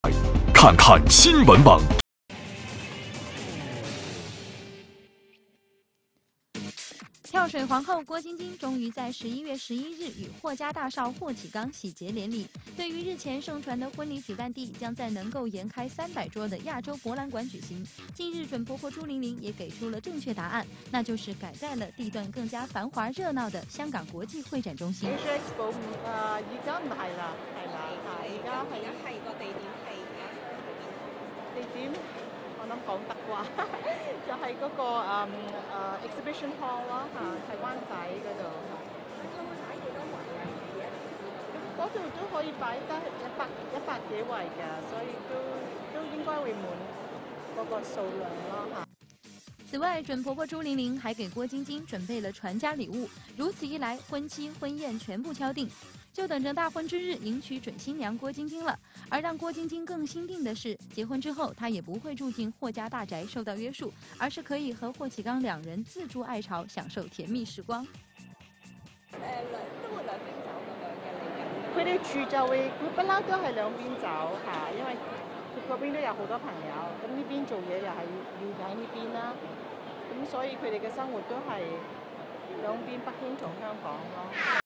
最心定：郭晶晶 "光棍节"大婚成定局
钻戒 前段时间,郭晶晶在英国名店LucieCampbell 选购3克拉钻戒,售价11万英镑。LucieCampbell 珠宝店有五十年历史,出品的古典钻饰在行内久负盛名,其三角形刻面的梨形钻石切割技术,更是独步世界。